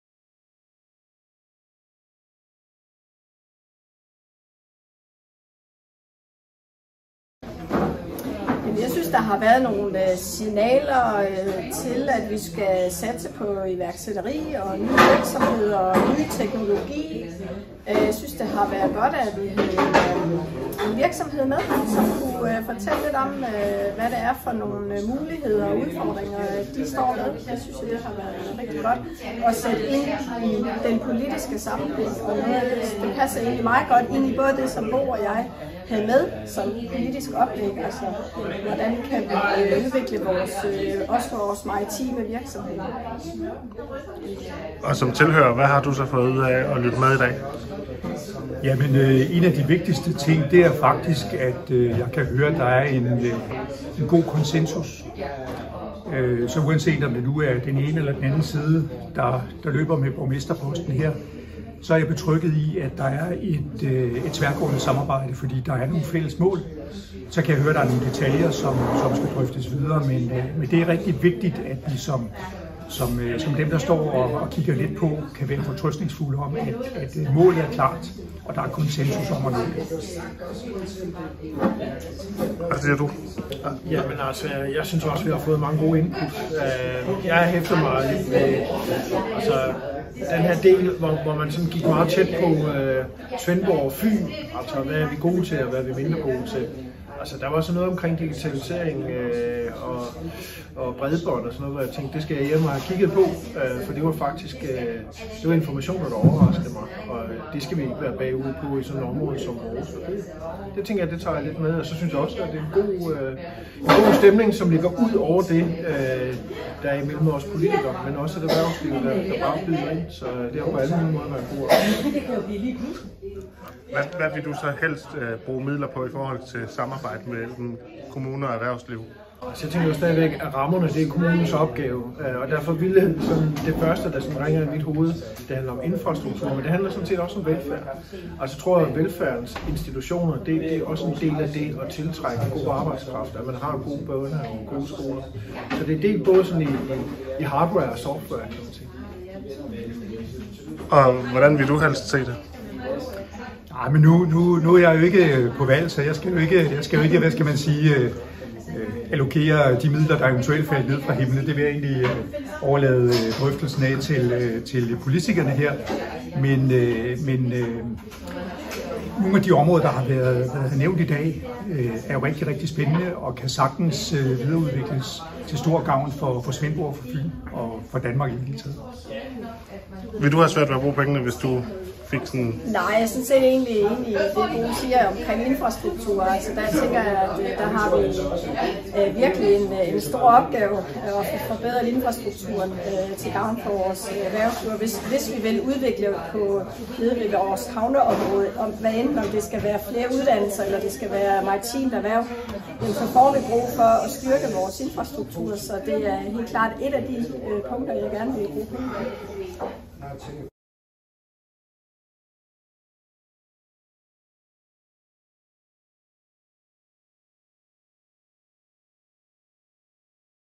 Tværgående samarbejde glæder i Svendborg
Bred enighed om, at en bedre infrastruktur og øget satsning på velfærd skal være med til at løfte erhvervslivet i Svendborg i fremtiden.